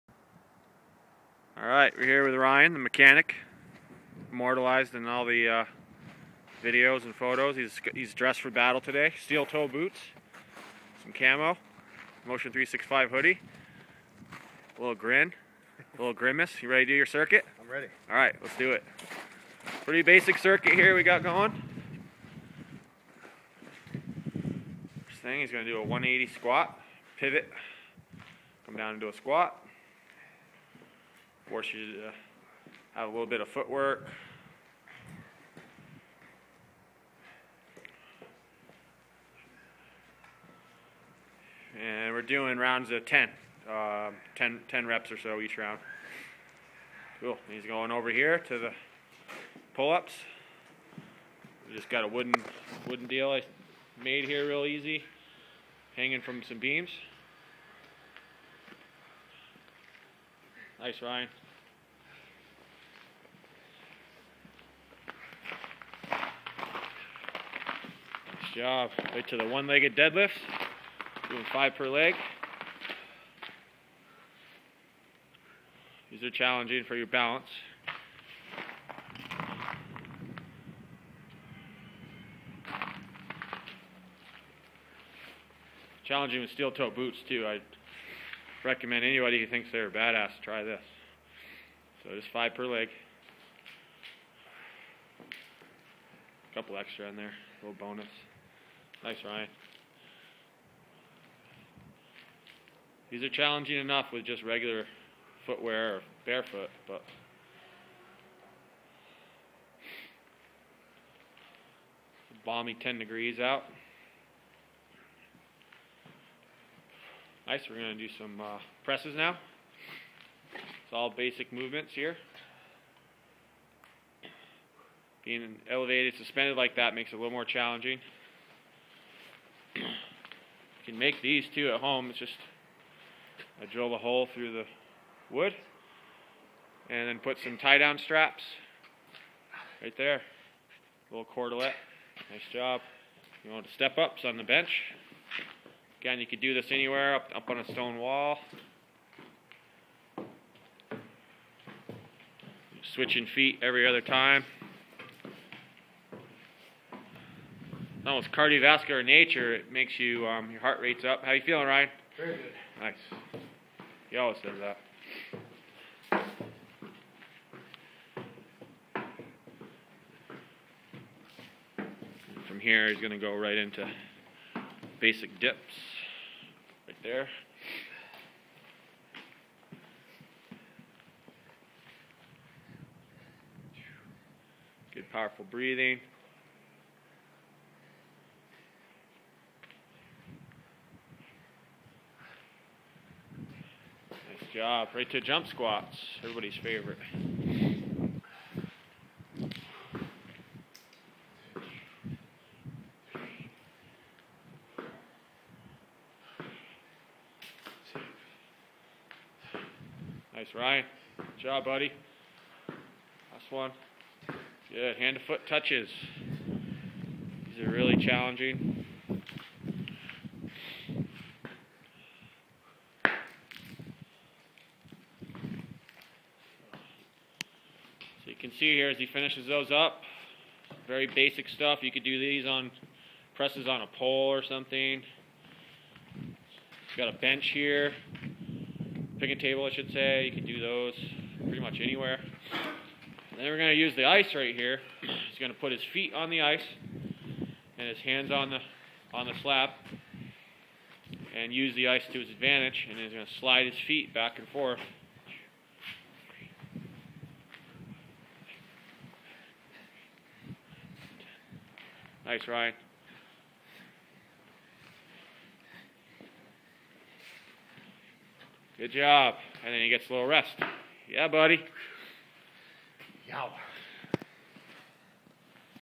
A great basic circuit, no equipment needed and not much time if your 're pressed for it.
Here is a short lo res clip of Ryan(the mechanic) demonstrating a basic full body circuit that you could easily do at home.